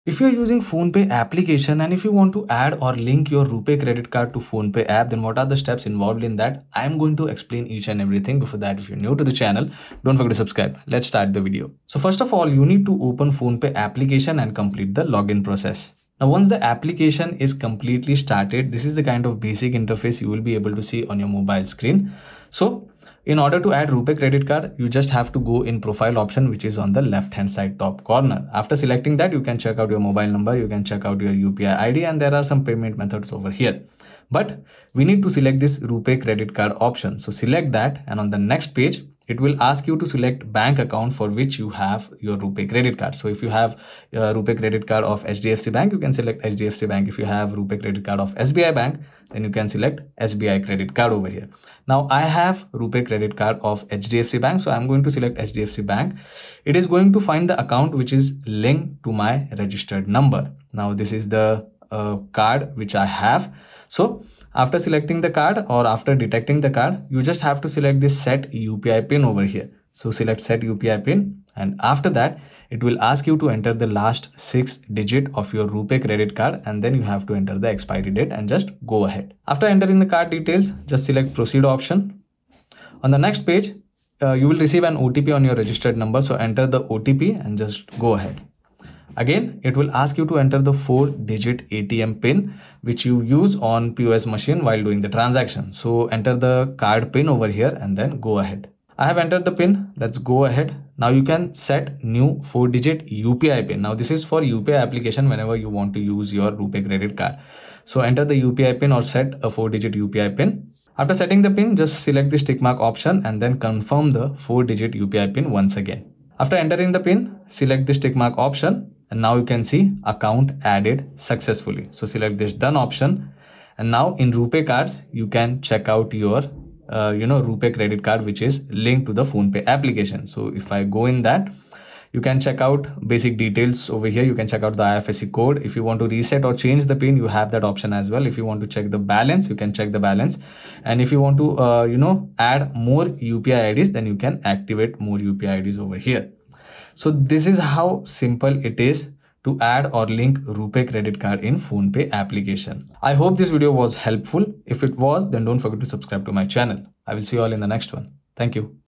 How to Link or Add the Rupay Credit Card to the PhonePe Application?
Learn how to add or link an HDFC Rupay credit card to the PhonePe application. Add your Rupay credit card to PhonePe in a few minutes. To add or link the Rupay credit card to the PhonePe application, open the PhonePe application on your mobile, log in to your account, and follow the steps given in the video.